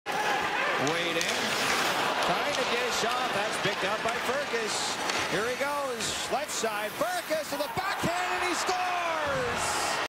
May 31 - Goal by Jagger Firkus
Jagger Firkus ties this one at ones.